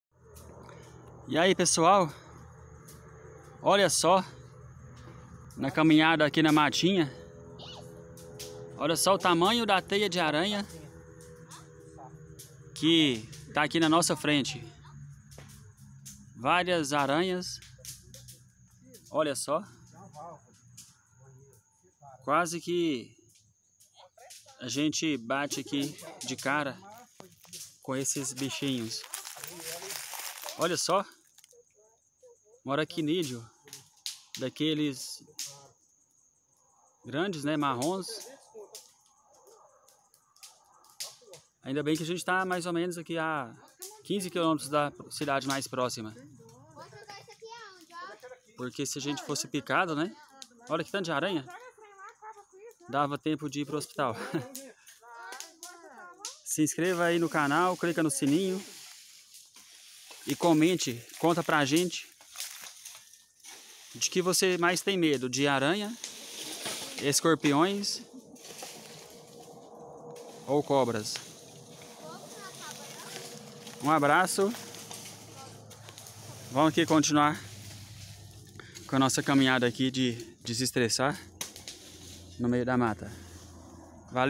As MAIORES Aranhas de Bananeira DO MUNDO. (Giant spider) Quase trombei na TEIA delas.
aracnídeos